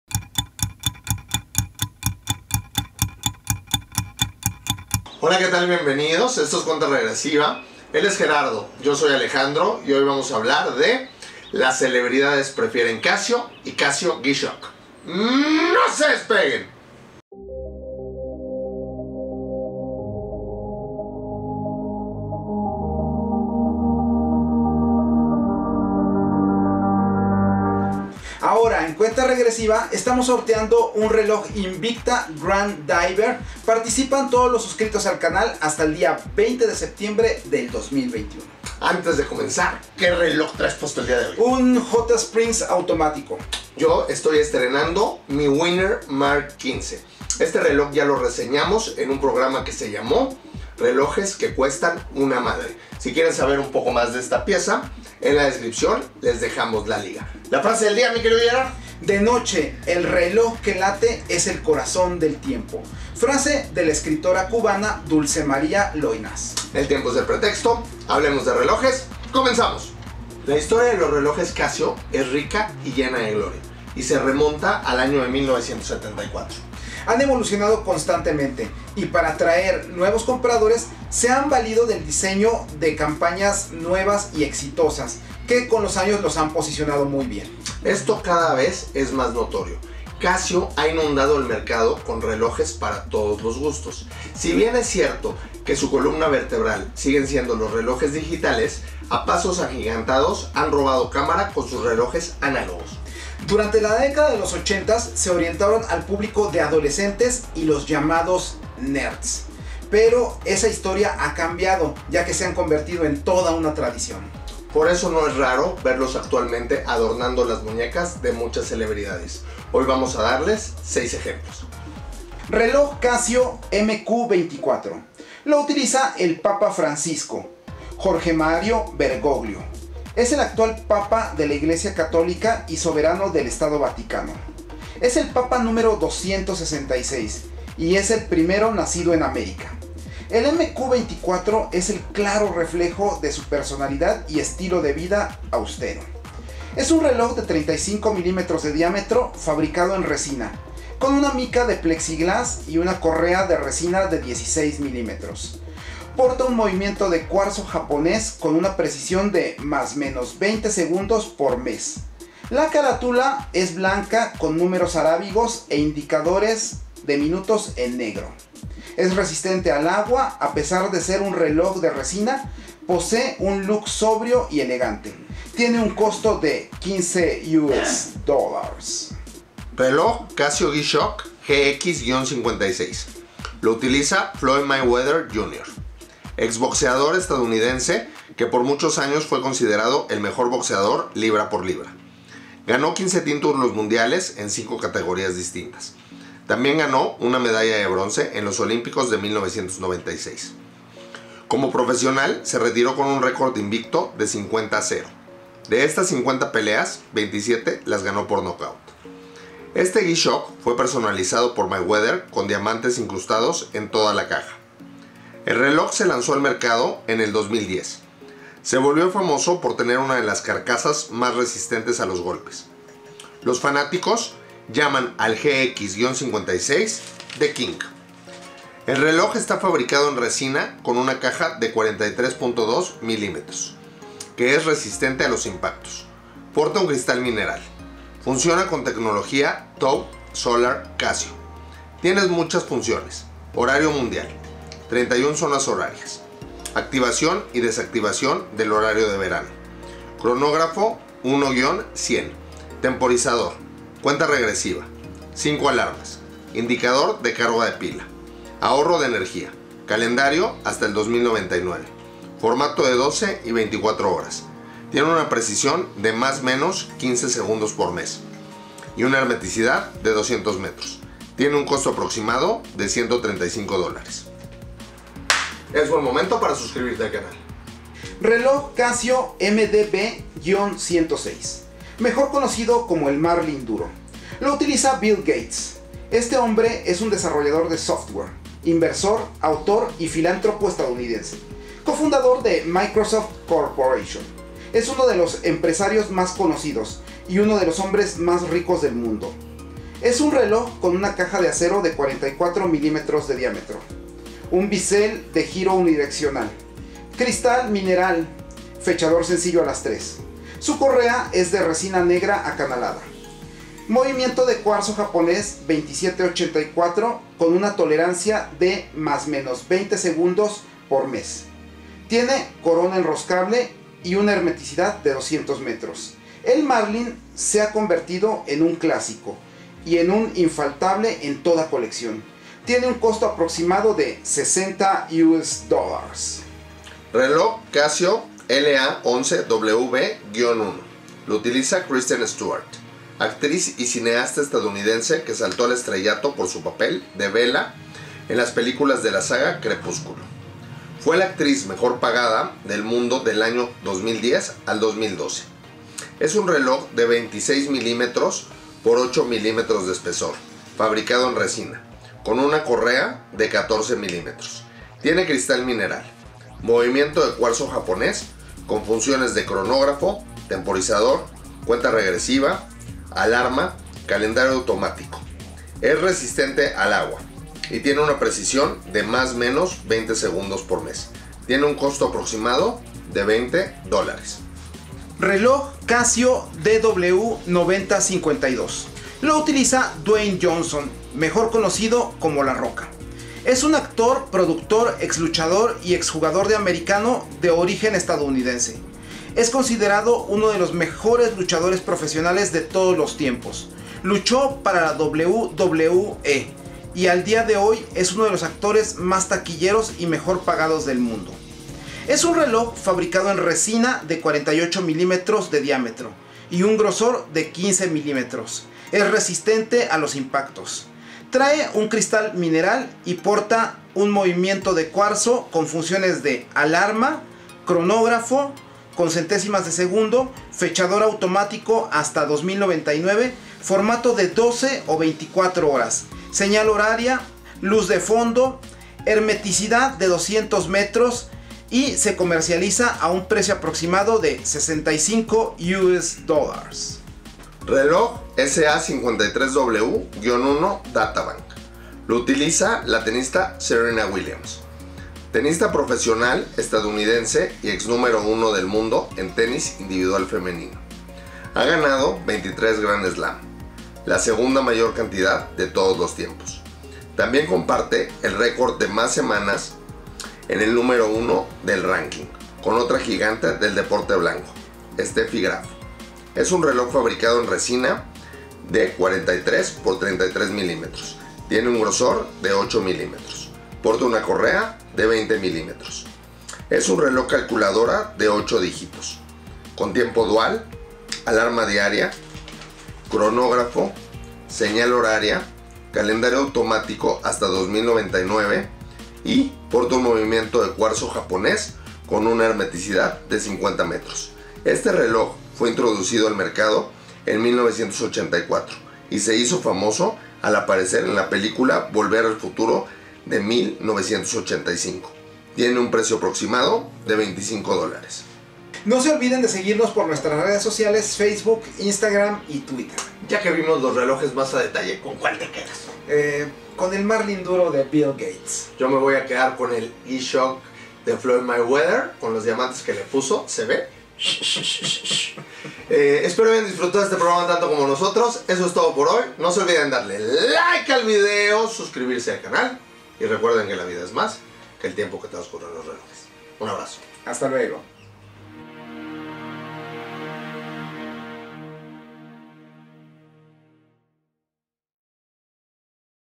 Las celebridades prefieren Casio y G shock, el Casio Mq del Papa Francisco, el Marlin de Bill Gates
🎁Vamos a sortear un Invicta Grand diver
solo tienes que estar suscrito al canal para participar

Les dejamos la liga de relojes chinos que cuestan una madre

Hoy Gerardo y Alejandro nos compartes los relojes Casio de 6 celebridades

El vida es más que el tiempo que transcurre en los relojes

UN ABRAZO